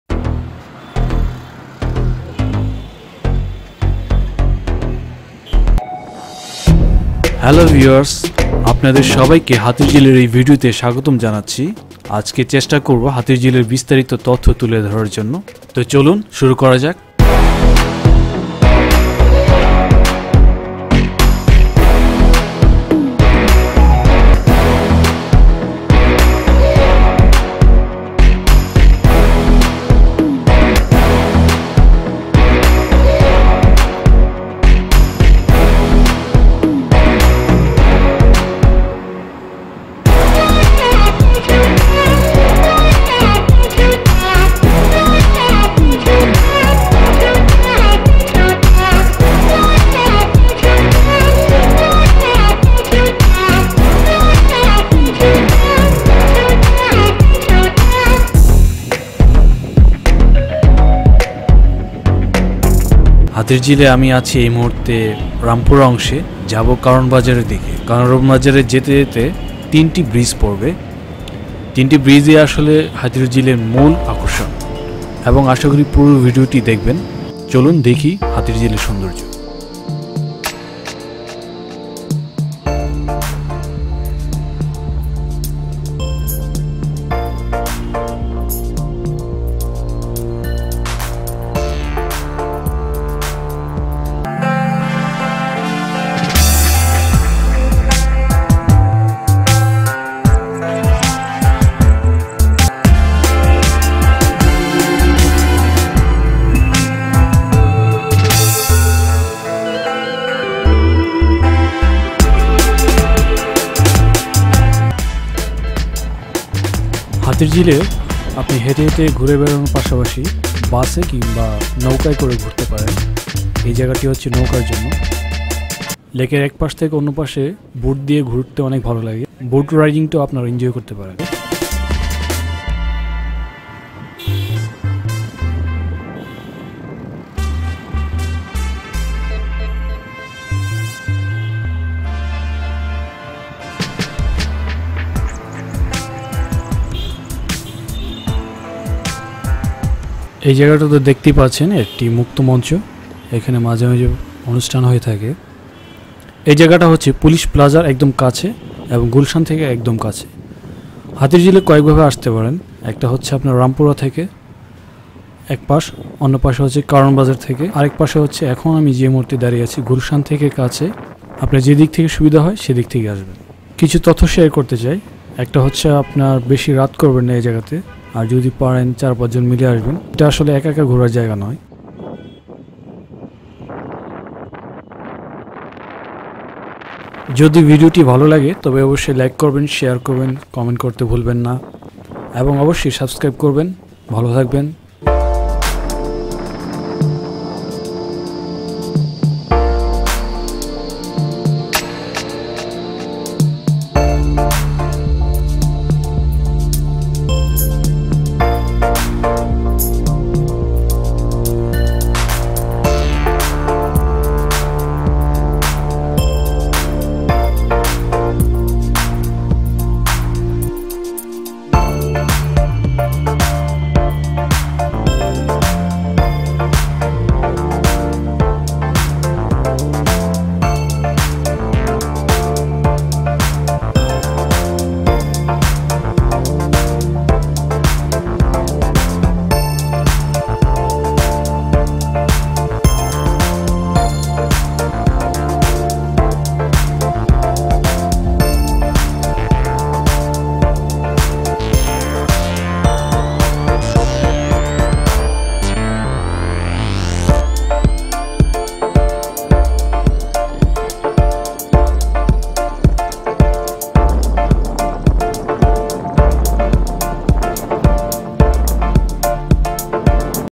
Hatirjheel Documentary. Entire Beautiful Hatirjheel Lake Drive - Bangladesh
Hatirjheel is a lakefront in Dhaka, Bangladesh. Government of Bangladesh has been transformed it into a popular tourist destination in Dhaka.